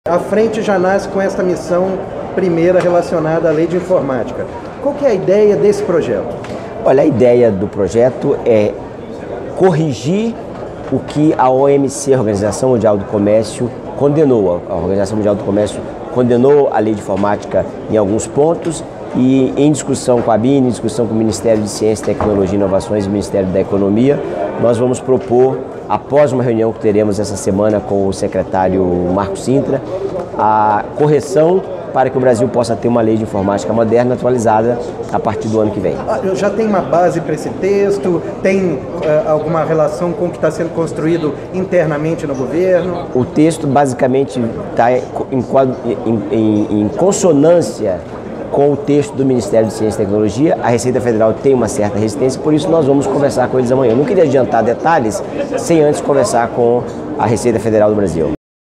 Sem Medida Provisória, deputados articulam projeto sobre Lei de Informática
À espera de uma Medida Provisória com mudanças na Lei de Informática (8.248/91), mas que ainda carece de entendimento interno no governo, deputados e representantes da indústria articulam um projeto de lei que enderece o assunto, como explica o o deputado e ex-ministro de Indústria e Comércio, Marcos Pereira (PRB-SP).